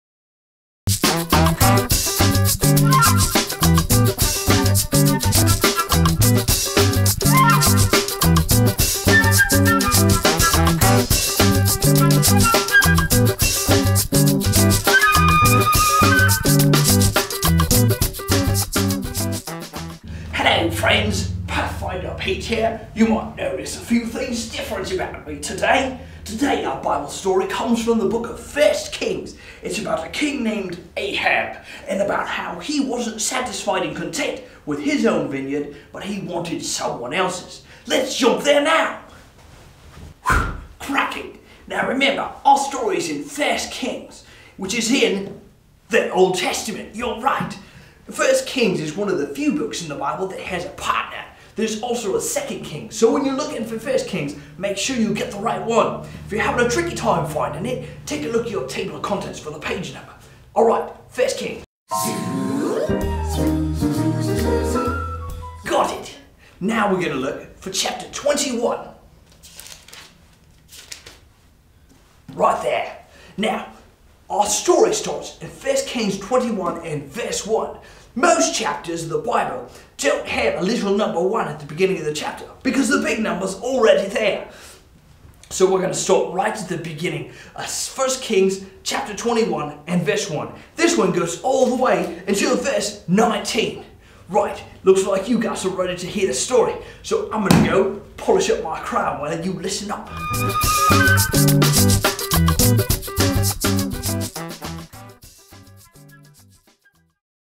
Pathfinder Pete episode 3
Pathfinder Pete talk about the story of King Ahab and Naboth's vineyard from 1 Kings 21.